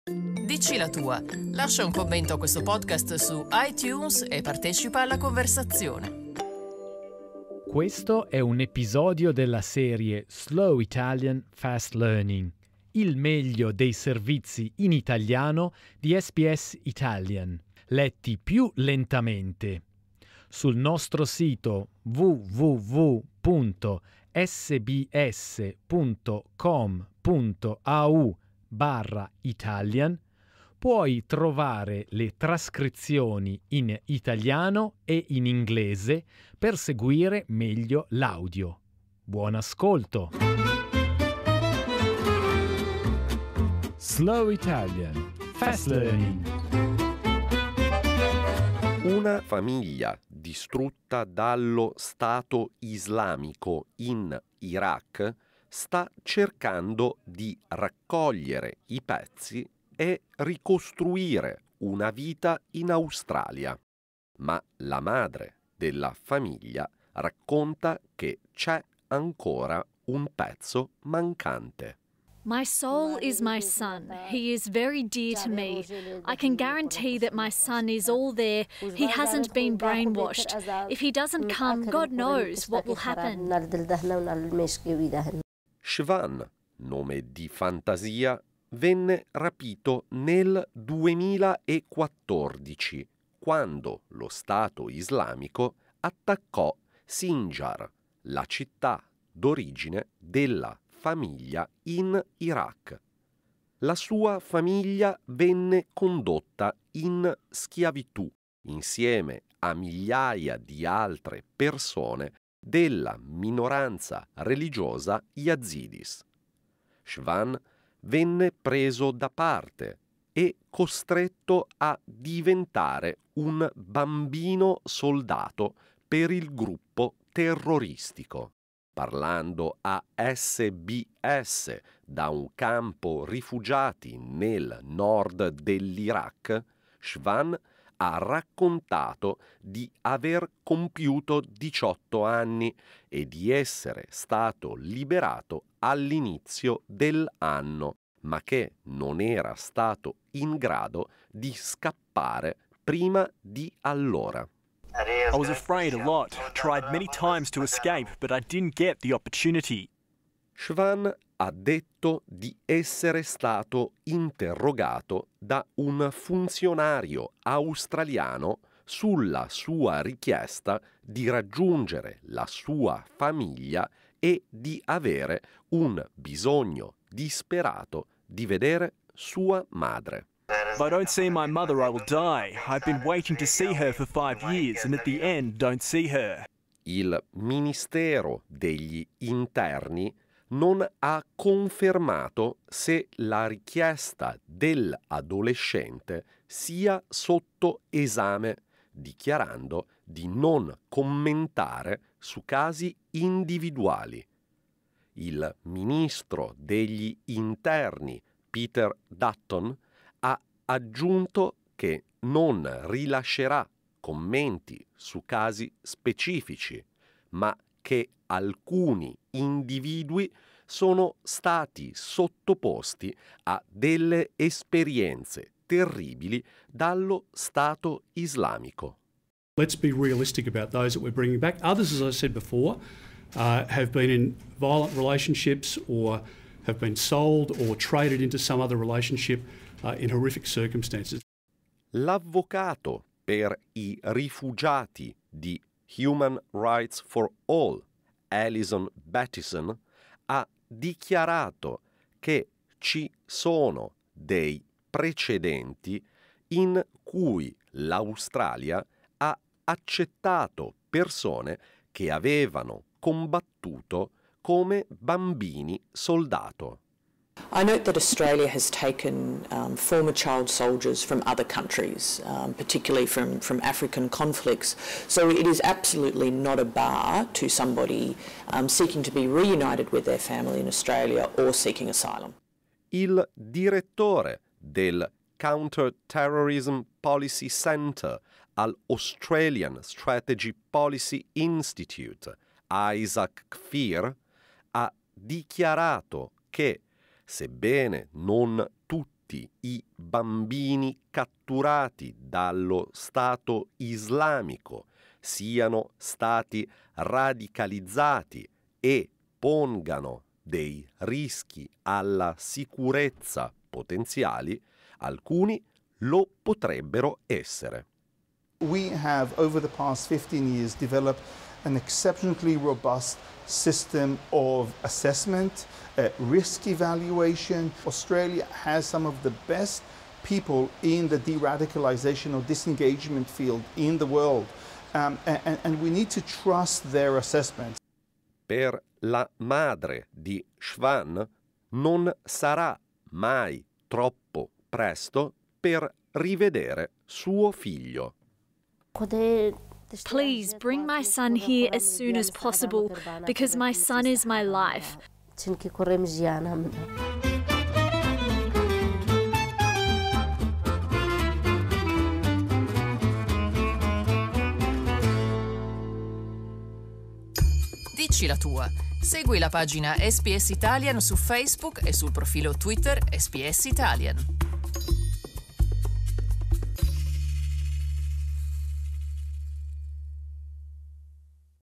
Episode 72: Government Considering Visa for Teen who Fought with IS - Episodio #72: Il governo co...
The government is understood to be considering a visa application from a Yazidi teenager, who was kidnapped and forced to fight with Islamic State. - Il governo starebbe considerando una richiesta di un visto da un adolescente Yazidi, che è stato rapito e costretto a combattere con lo Stato Islamico.